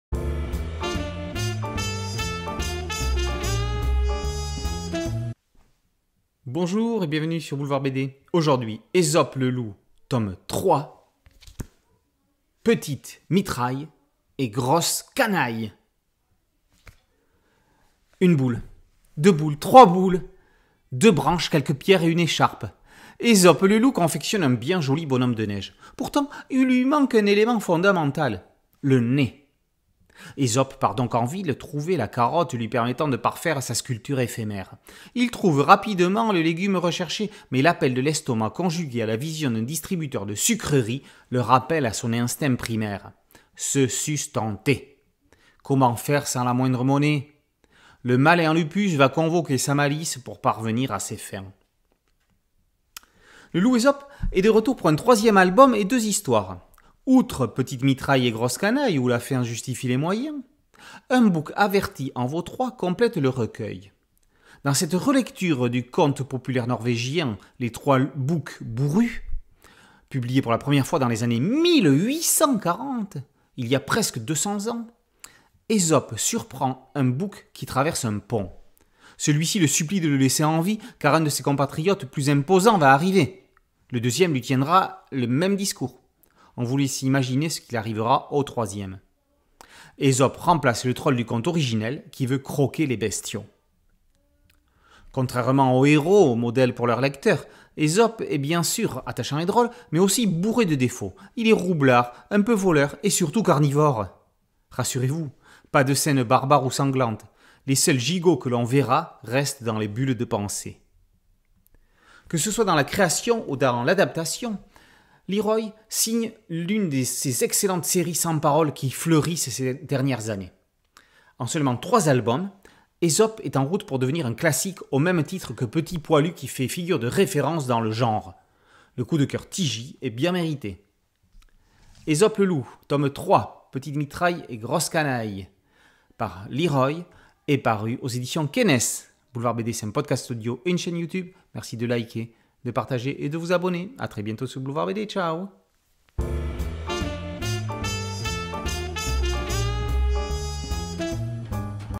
Esope le loup 3
Une boule, deux boules, trois boules, deux branches, quelques pierres et une écharpe. Esope le loup confectionne un bien joli bonhomme de neige. Pourtant, il lui manque un élément fondamental : le nez. Esope part donc en ville trouver la carotte lui permettant de parfaire sa sculpture éphémère. Il trouve rapidement le légume recherché mais l’appel de l’estomac, conjugué à la vision d’un distributeur ,de sucreries le rappelle à son instinct primaire : se sustenter. Comment faire sans la moindre monnaie ? Le malin lupus va convoquer sa malice pour parvenir à ses fins.

Laurent Lafourcade

Série : Esope le loup

Tome : 3 - Petite mitraille et grosses canailles

Genre : Humour animalier

Scénario, Dessins & Couleurs : Liroy

Éditeur : Kennes

Nombre de pages : 32

Prix : 9,95 €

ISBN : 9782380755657

© Liroy - Kennes